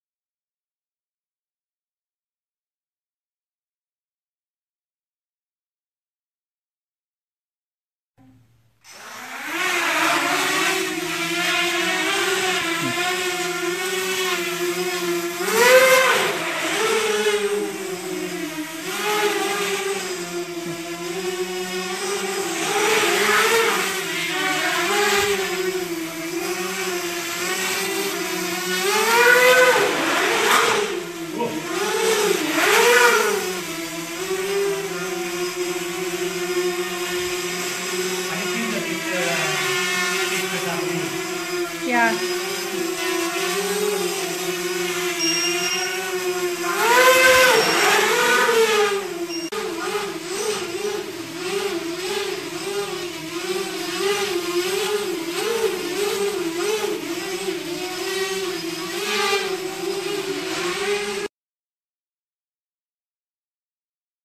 TILT PRO v2 First tests (4S)
First tests of the new TILT PRO v2 (in 4S, 5S/6S tests on going and publishing as soon as we finish... damn fast though). This new version is faster, lighter, tougher and with less parts. We do have a launch offer in the link below, check it out.

Happy flying!